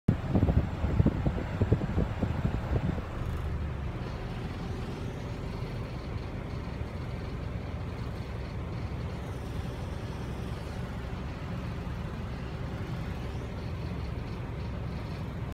I was the only one in this minibus (I wasn't supposed to be there)
I didn't know I had to get off and get on the same bus route. I had to do so for some reason.